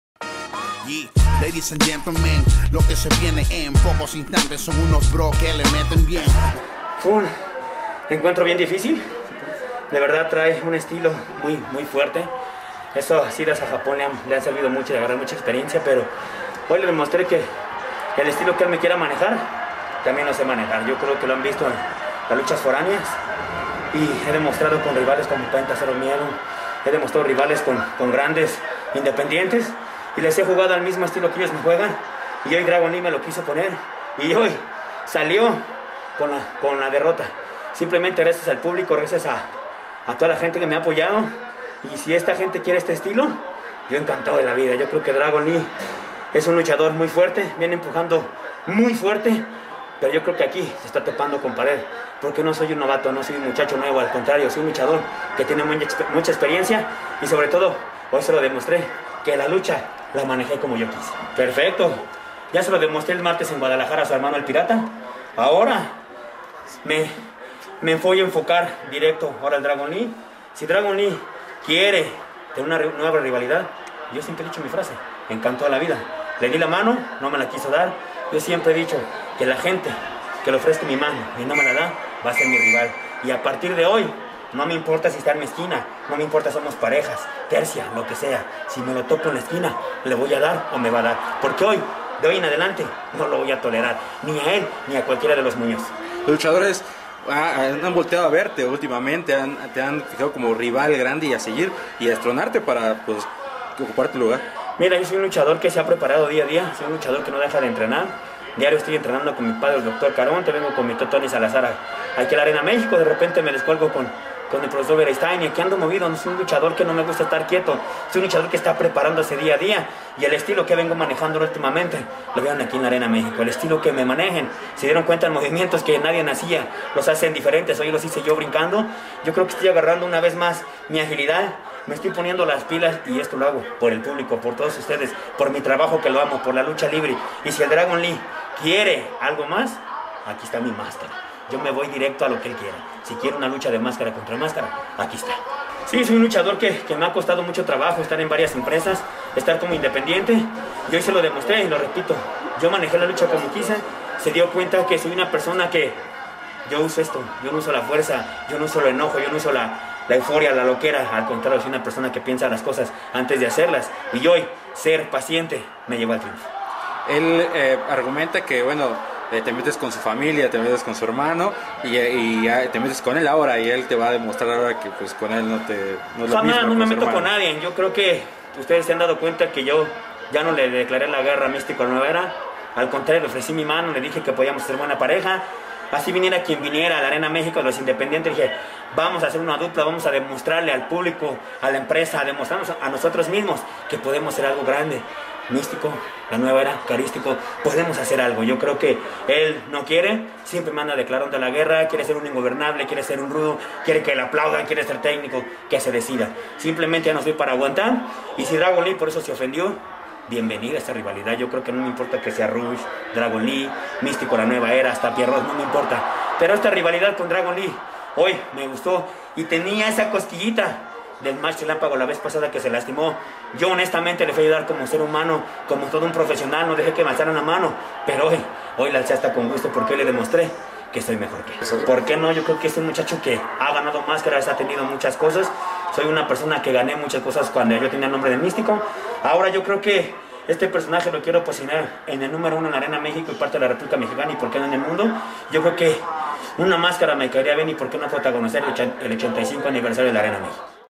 Si Dragon Lee quiere una lucha máscara contra máscara, aquí está: Carístico
Carístico vence a Dragon Lee dentro de un match relámpago; el Original de Príncipe de Plata y Oro está dispuesto a todo ante el menor de la Dinastía Muñoz.